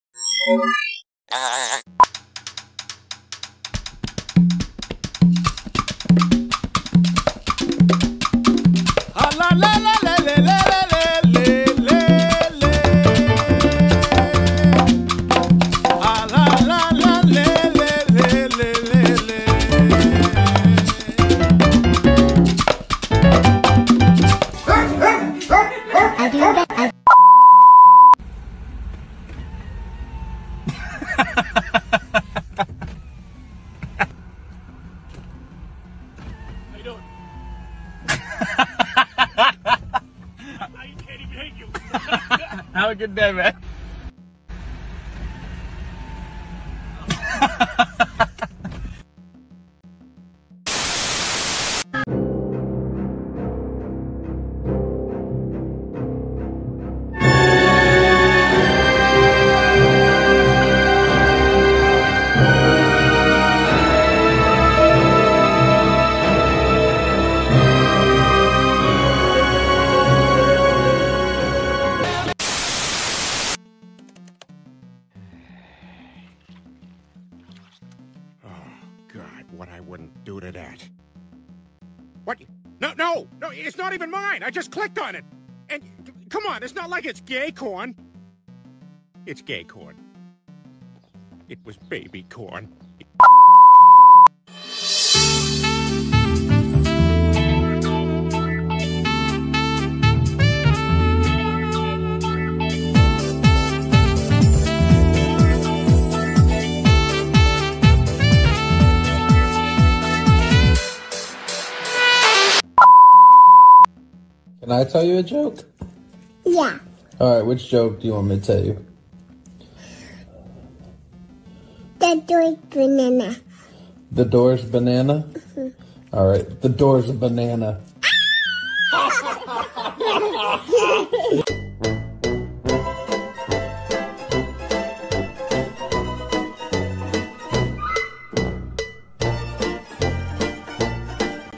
The Best Memes on the Internet!! #016
Looking for some of the funniest and most popular memes on the internet? In this video, we've compiled a list of the best meme shorts that have been circulating online. From classic memes to the latest trends, this video has something for everyone. Whether you're a meme enthusiast or just looking for a good laugh, make sure to watch this compilation of the funniest memes!